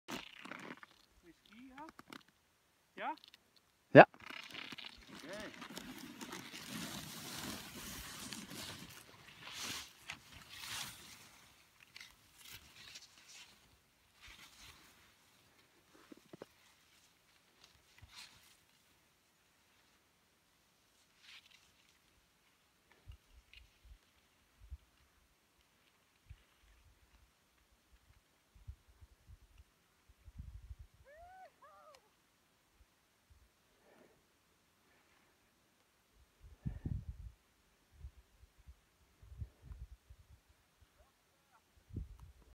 Schöffel Ski Sunshine: Rainer und Bart Goateye Mountain (März 2016)
SK Chef Rainer und Bart wagen sich den Goateye Mountain hinab.